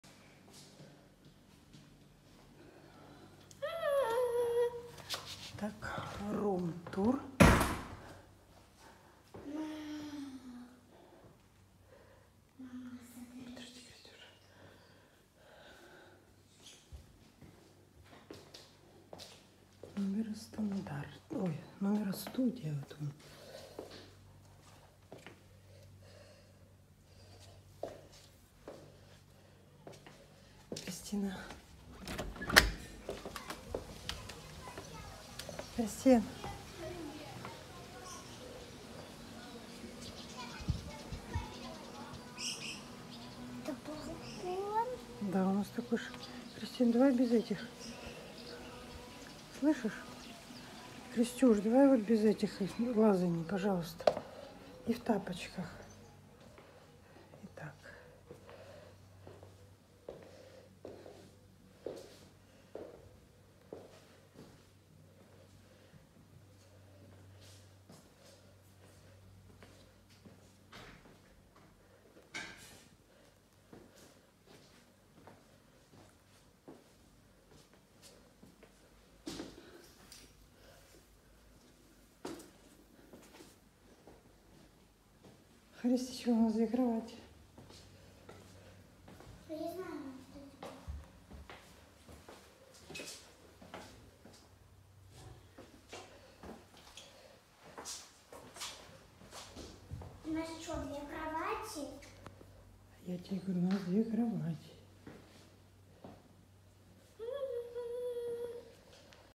Румтур номер Студия база отдыха Ладога
Мы проводим с дочей отпуск в Анапе. Будем показывать и рассказывать интересные моменты.